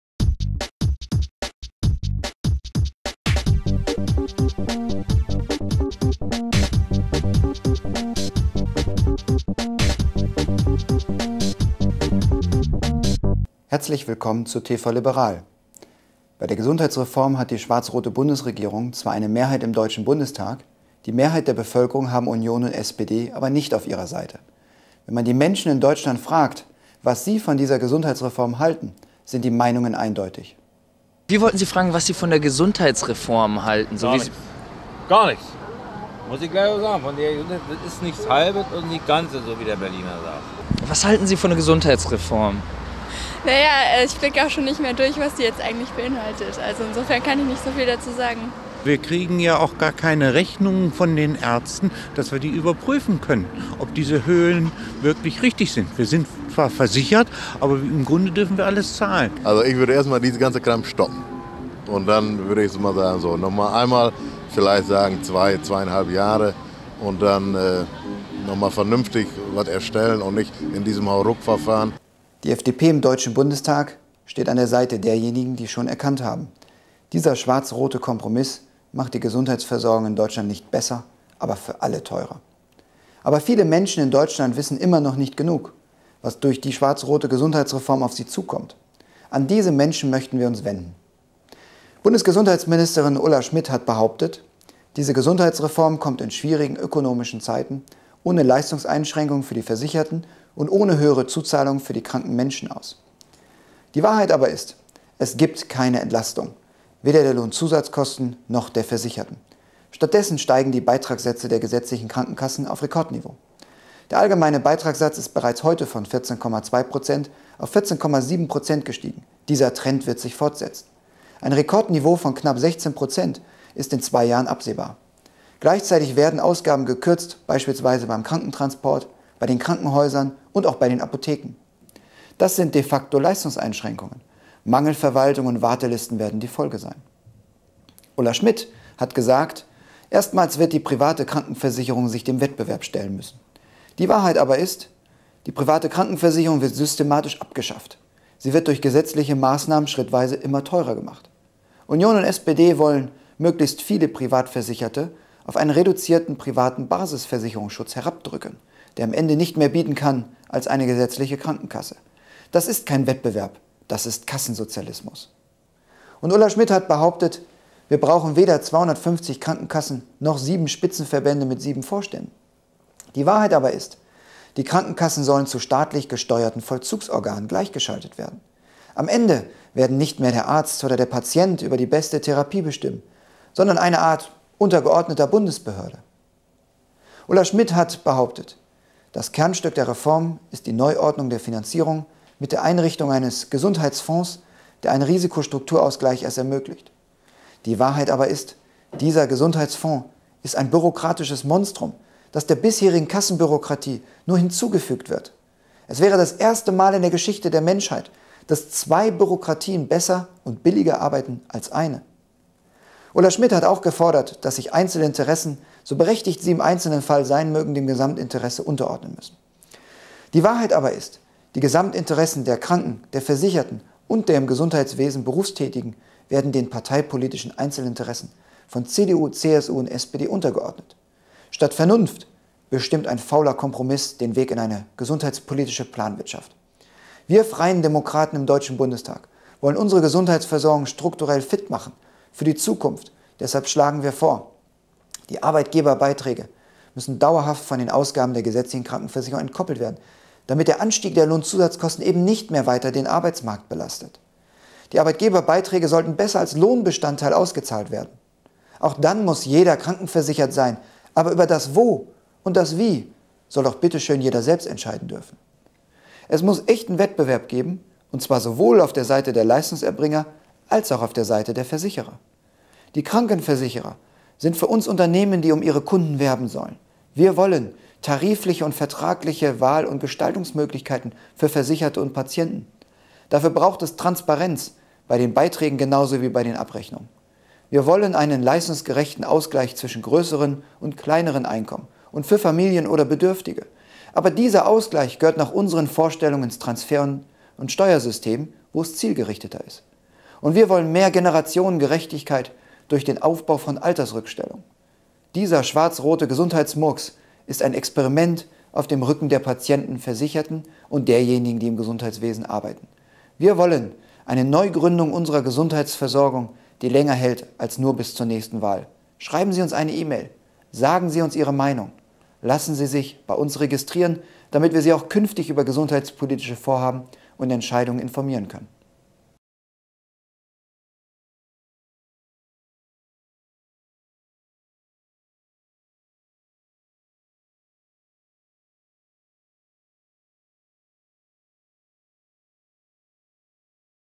Daniel Bahr zur schwarz-roten Gesundheitsreform
Der gesundheitspolitische Sprecher der FDP-Bundestagsfraktion, Daniel Bahr, kritisiert die schwarz-rote Gesundheitsreform.